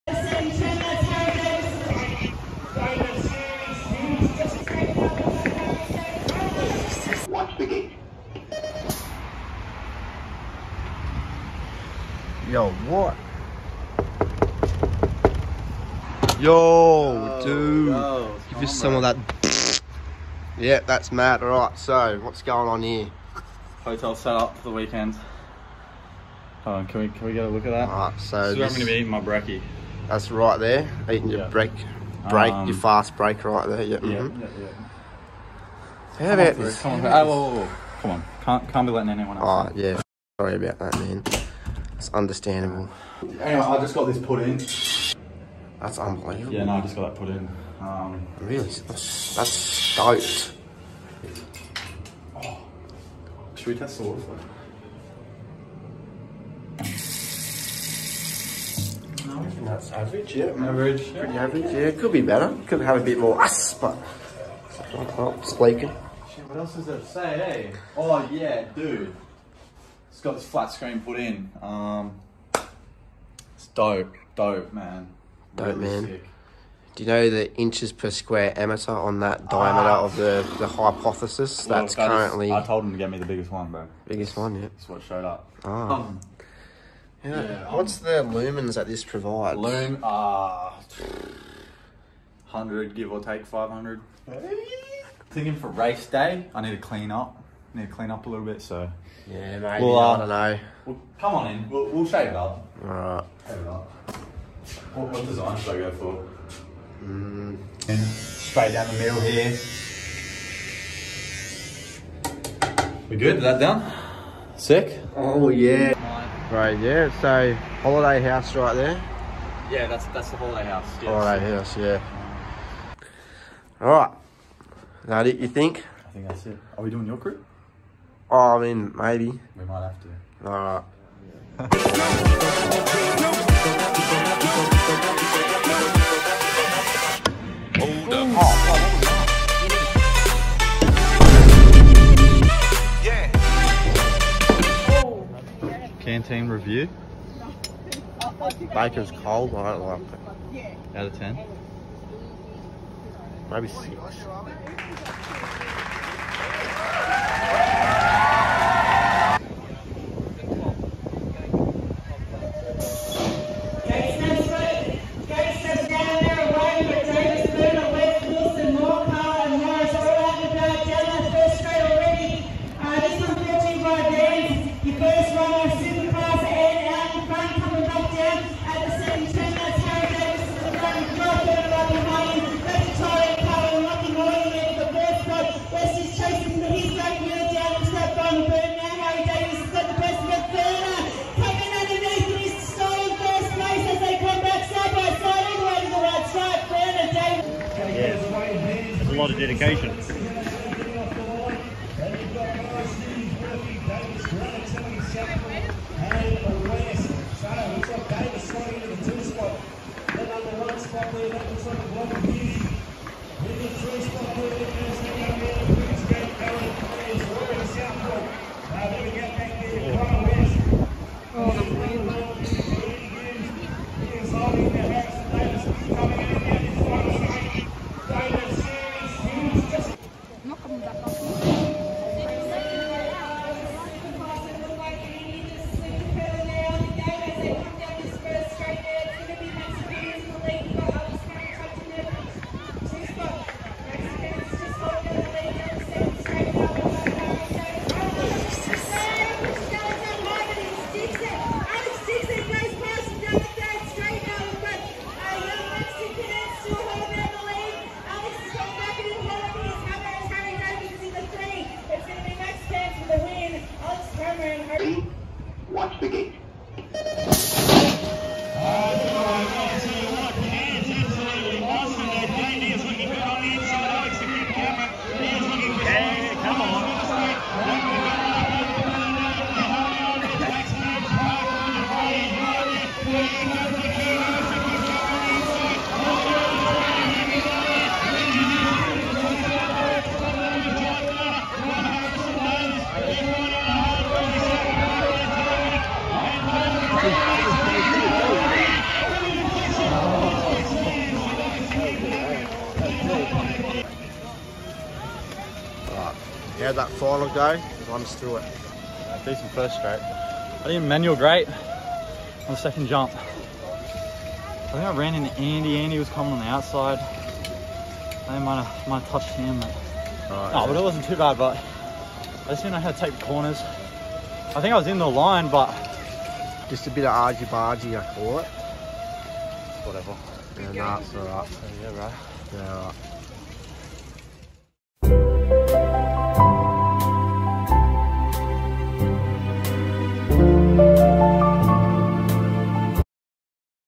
Manly BMX State Series Round Eight | Harrison Davis
Last round of the NSW state series. skip to 2:30 for the racing action.